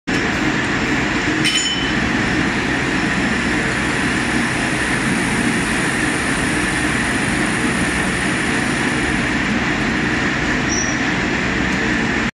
Zinc wire being produced ,Pure Zinc wire is being wrapped in spool /axis 99.995% purity
Thermal spray Pure Zinc wire /Arc spray wire /TWIN WIRE ARC SPRAY

It is used for Galvanized welded pipe /steel tube /capacitor/dockyard /steel structure /tower /Gas Cylinder corrosion protection

Diameter 1.2mm . 1.5mm ,2.0mm 3.17mm
Package : Bundle , Spool , Drum
Welcome inquiry